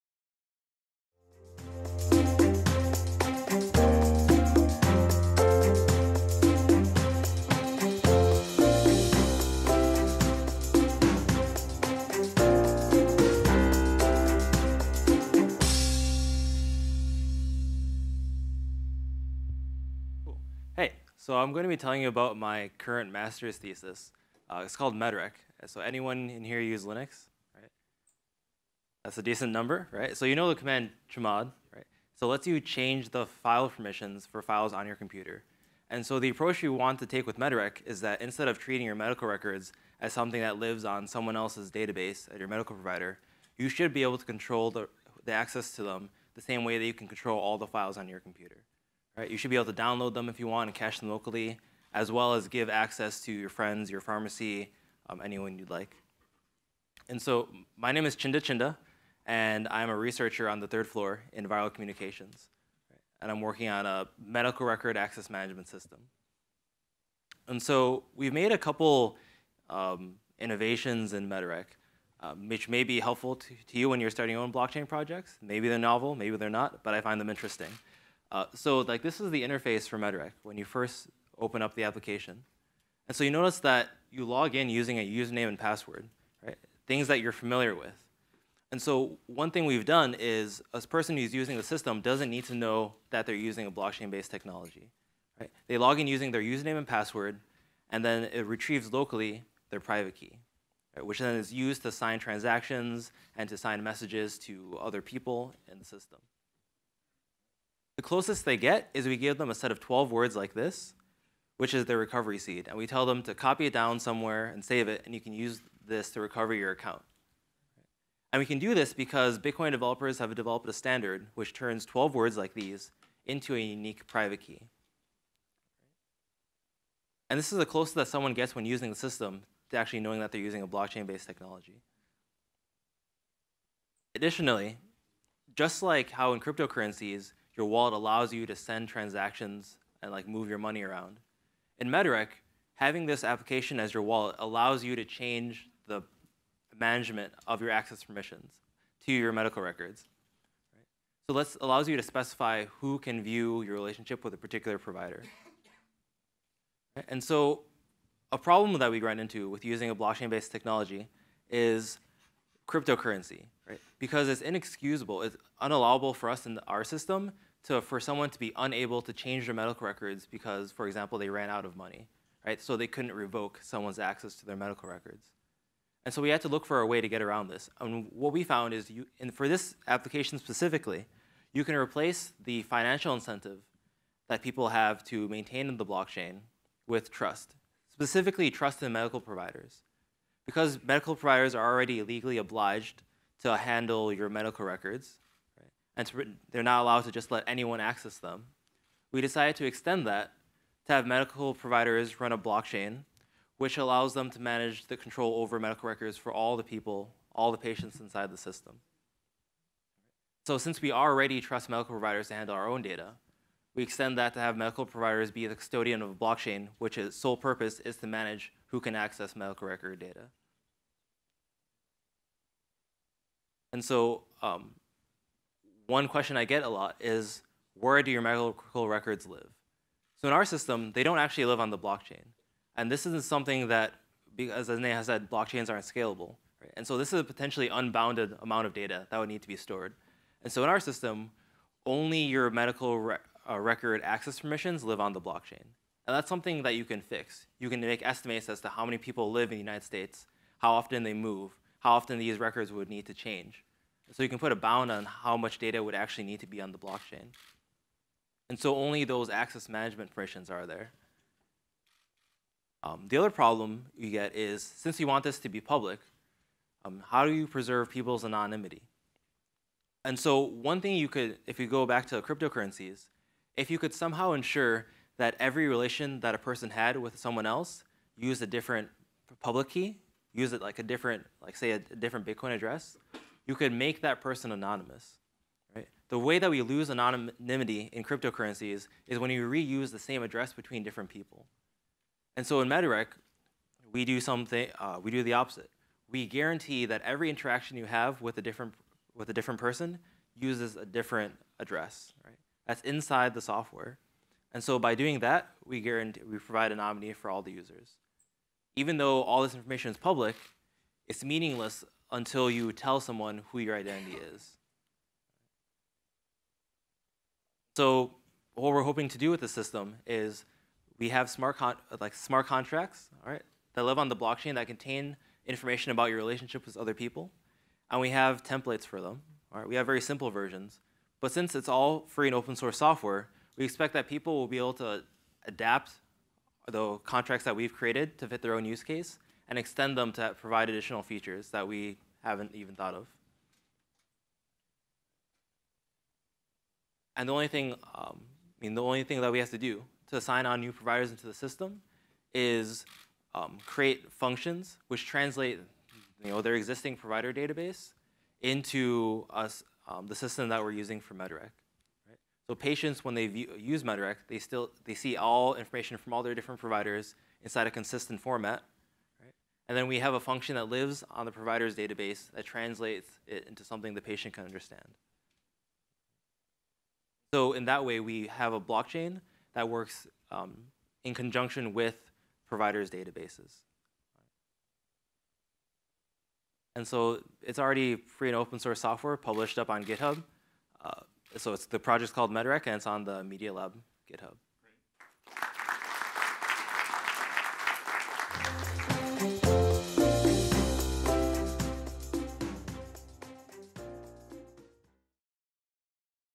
Take Back Your Medical Records with Blockchain | Nchinda Nchinda | Blockchain+AI+Human
Where are your medical records kept? Maybe with your general care provider, maybe you have some folders up in your filing cabinet. For his masters thesis from MIT, Nchinda Nchinda tells us about ways we can have personal, digital, and secure ownership over our medical records with MedRec. A program that operates using blockchain technology.

Nchinda Nchinda
I am going to the Massachussetts Institute of Technology to learn Computer and Political Science. I would like to work in Artificial Intelligece and/or CyberSecurity and/or Web/Mobile Development.

Specialties: Programming(Java, Javascript, Python), Web Development, playing the tuba and other brass instruments, learning at an accelerated rate, listening,